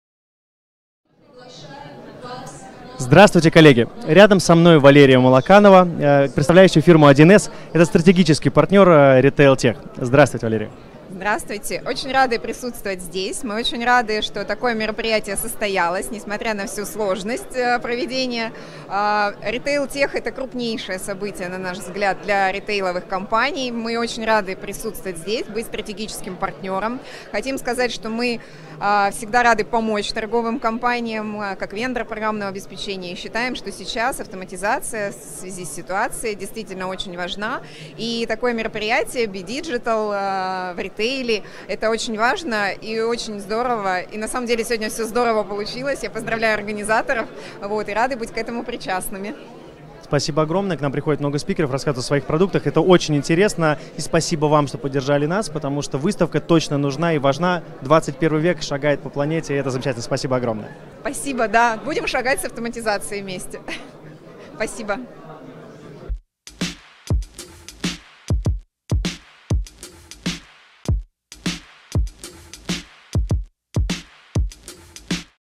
Валерия Молоканова, «1C», на
Валерия Молоканова, «1С», об автоматизации и впечатлениях от выставки